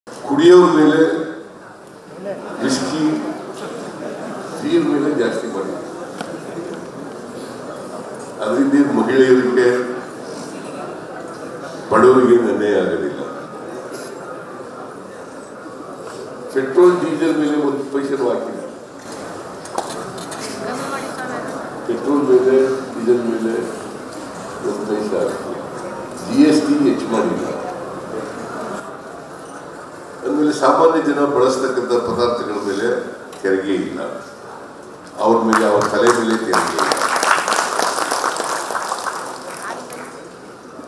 Siddaramaiah: ಎಣ್ಣೆ ರೇಟ್ ಜಾಸ್ತಿ.. ಸ್ಟೇಜ್​ ಮೇಲೆ ಕಾಮಿಡಿ ಮಾಡಿದ ಸಿಎಂ ಸಿದ್ದರಾಮಯ್ಯ!
Btv News Kannada:Siddaramaiah: ಎಣ್ಣೆ ರೇಟ್ ಜಾಸ್ತಿ.. ಸ್ಟೇಜ್​ ಮೇಲೆ ಕಾಮಿಡಿ ಮಾಡಿದ ಸಿಎಂ ಸಿದ್ದರಾಮಯ್ಯ!